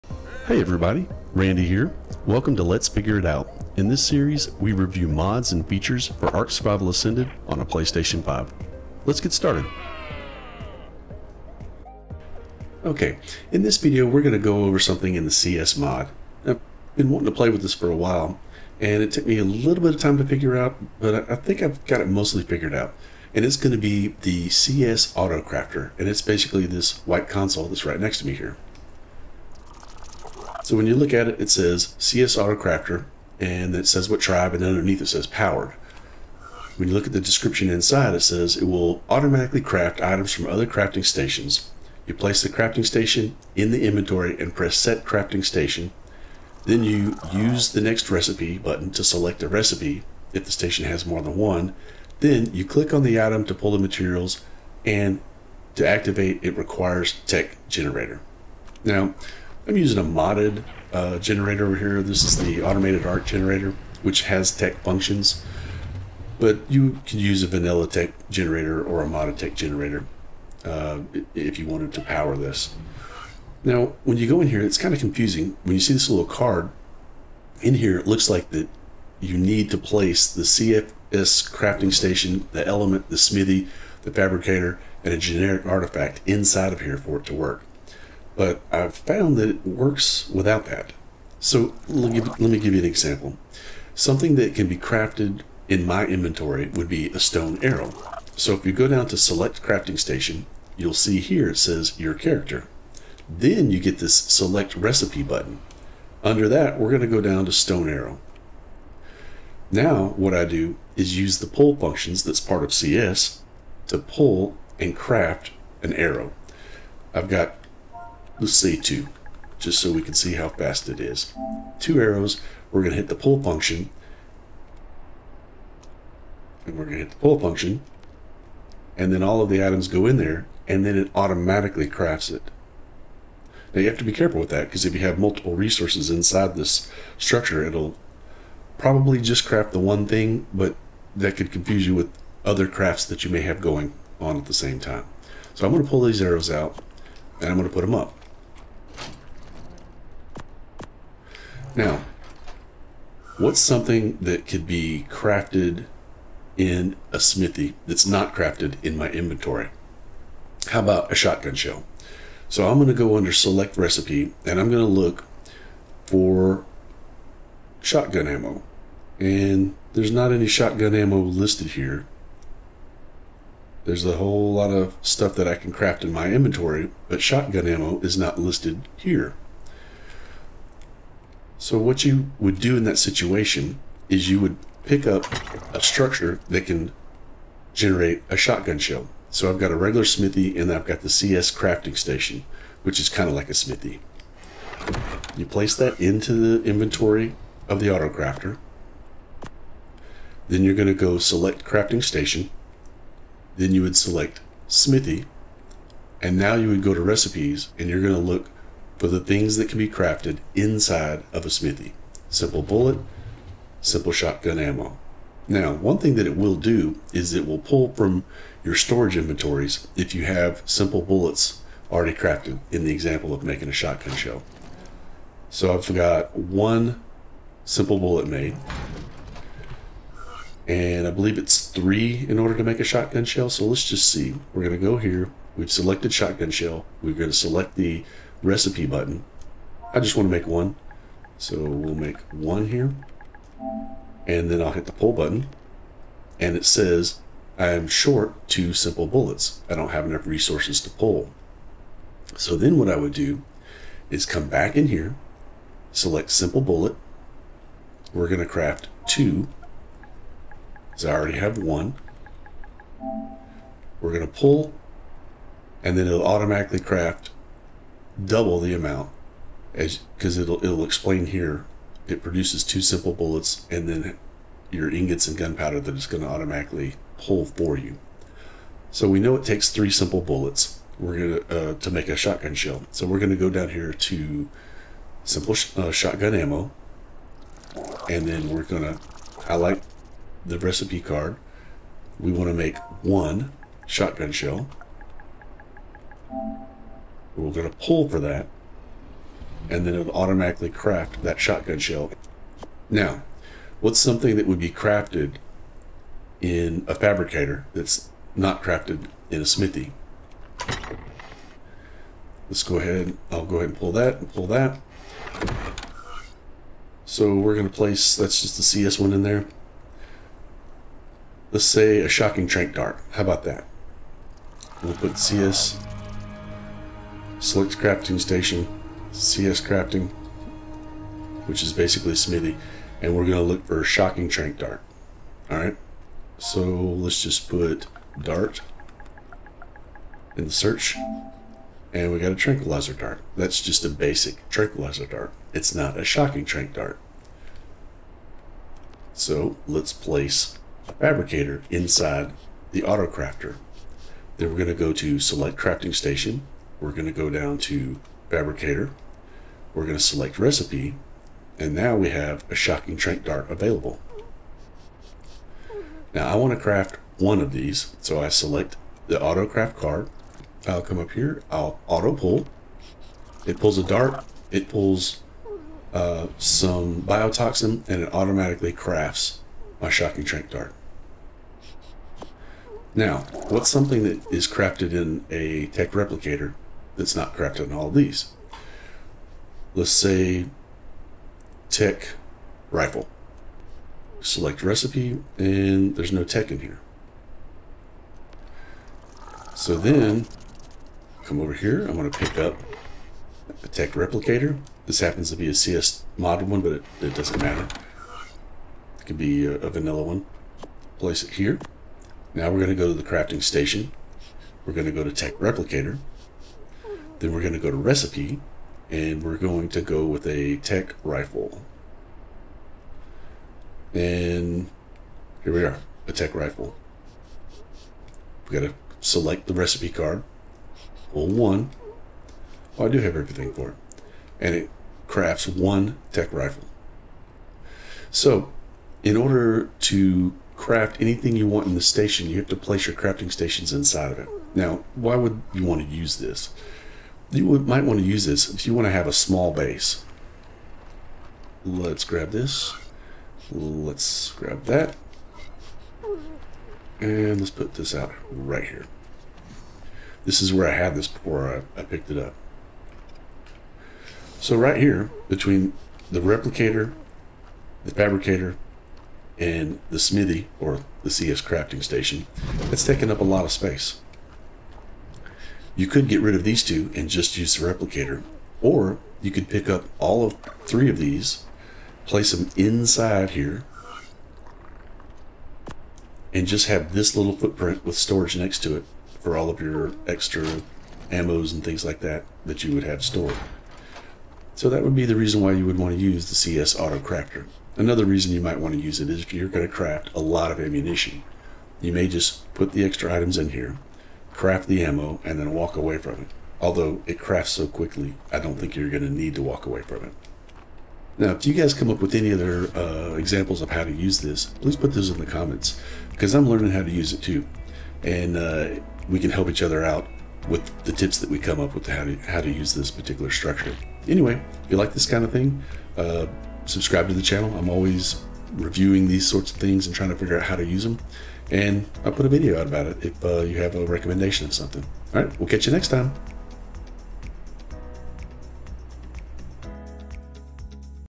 CS AUTO CRAFTER
In this video a breakdown how to use the Auto Crafter from the Cyber Structures Mod.